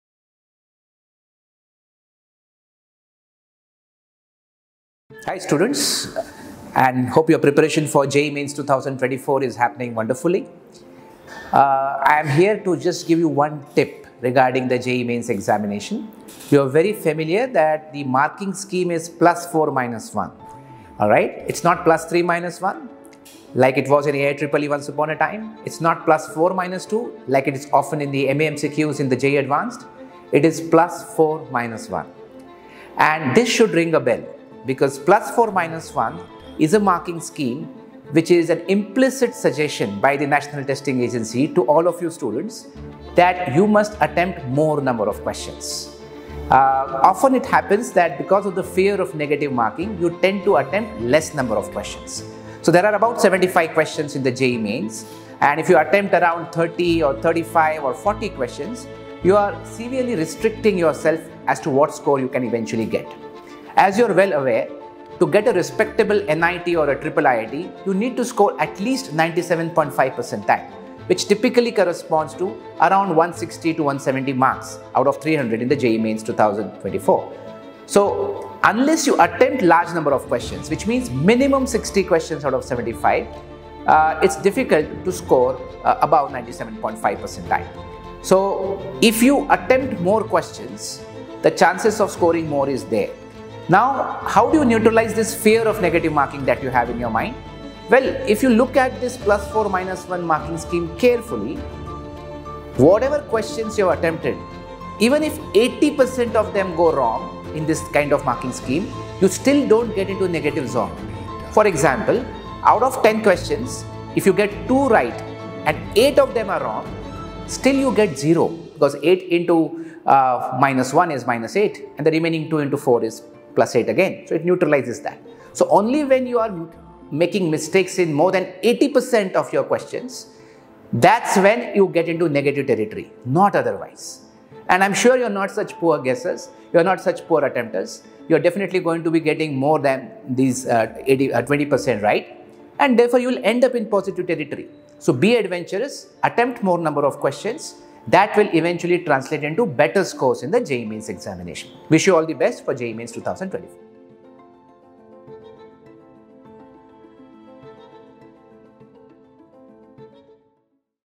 IIT JEE Exams - Negative Marking Decoded
When tackling the IIT JEE Exams, many students believe that attempting fewer questions and ensuring accuracy is the key, rather than taking risks with more attempts that might be wrong. Let's debunk some myths and share valuable tips from Professor Durgesh Mangeshkar, the Founder Director of IITPK.